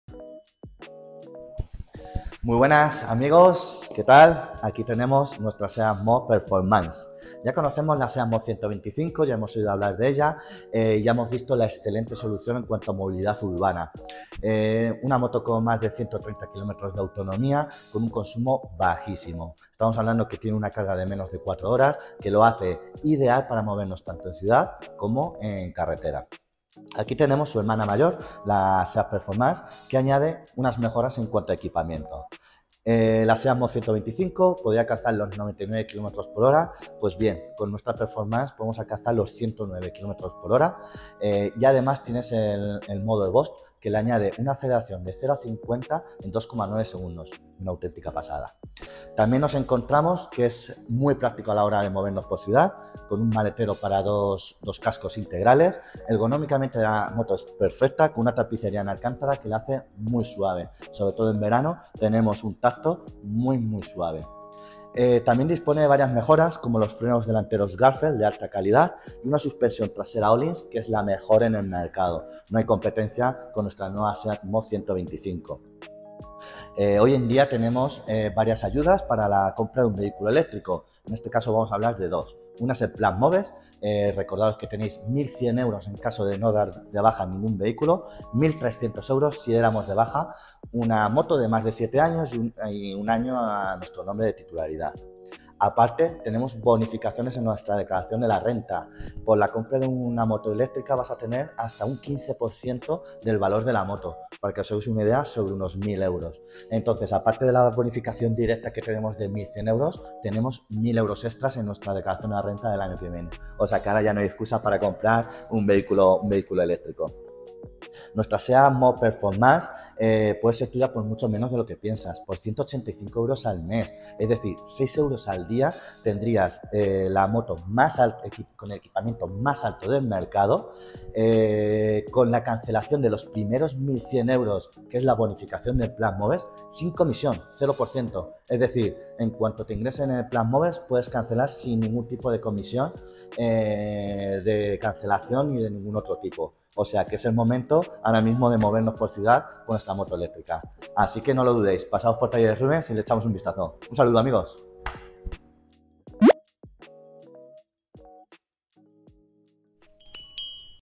SEAT MÓ Performance 💥
¡Hola amigos! En el vídeo de hoy nuestro asesor comercial Santi nos mostrará esta única scooter 🛵 100% eléctrica ⚡️

SEAT MÓ Performance con un precio inigualable➡️ 185€/mes*